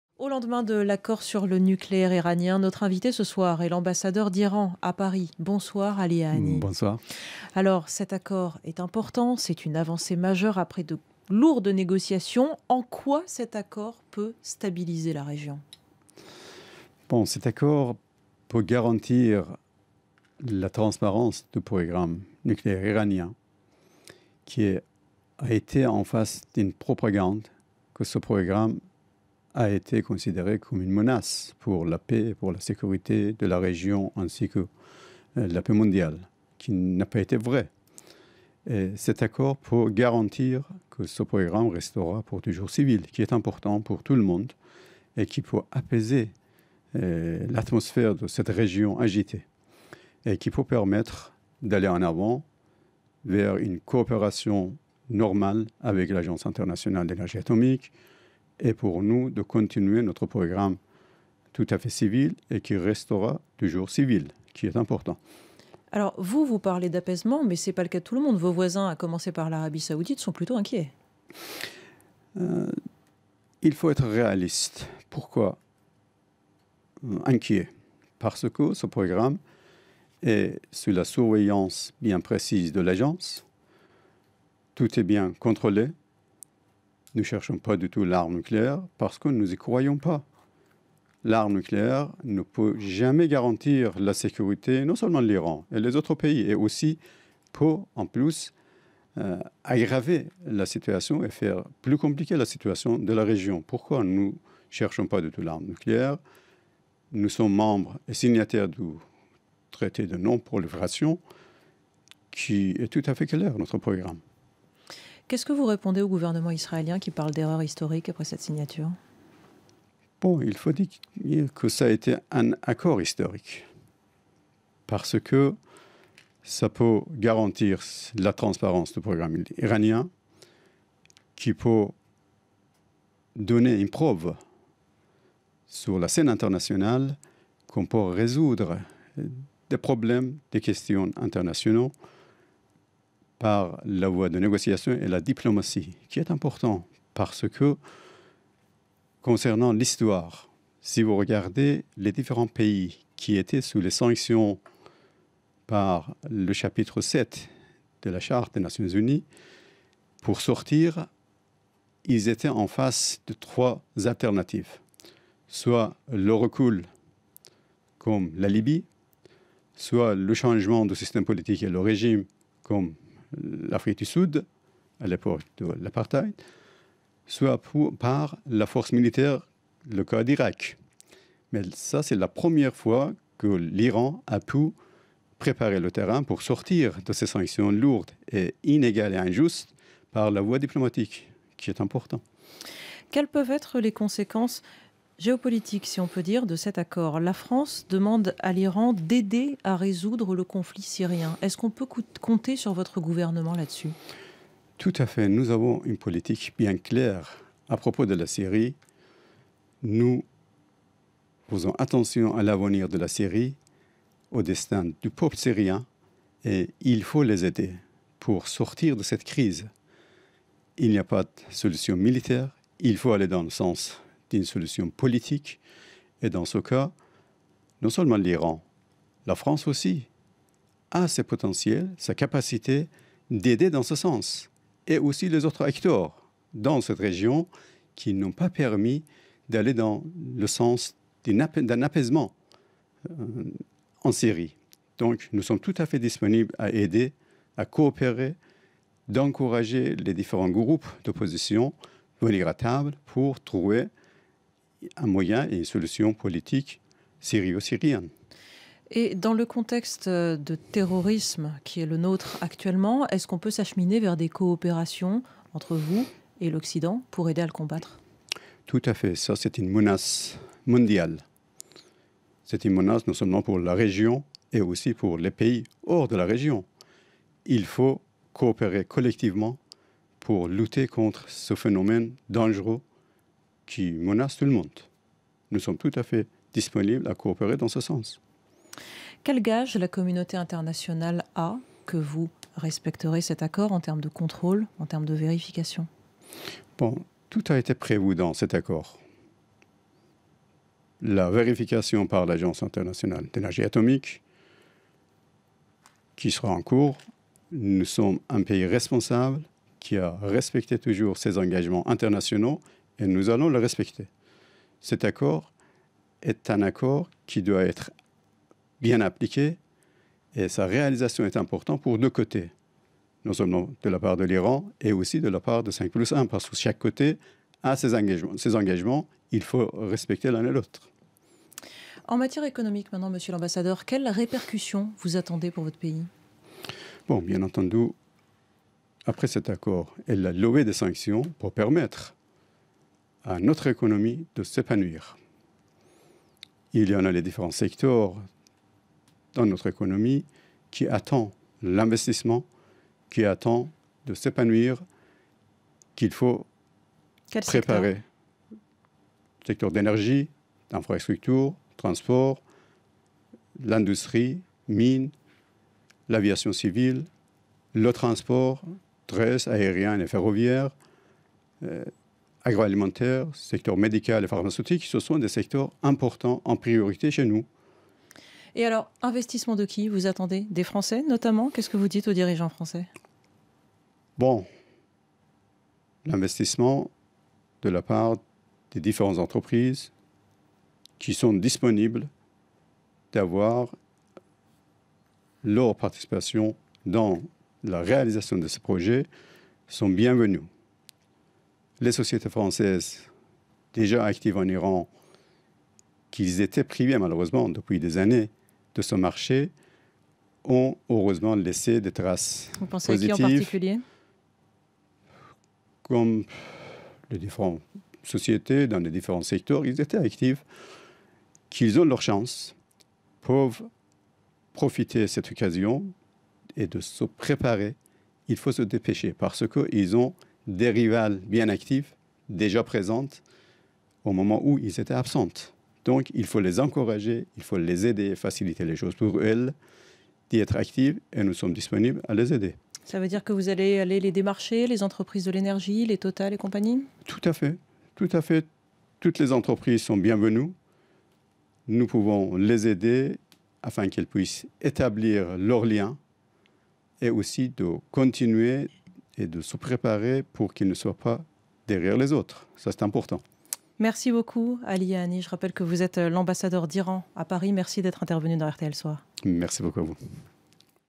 Ali Ahani, ambassadeur d'Iran, invité de RTL Soir, le 15 juillet 2015 - RTL - RTL
null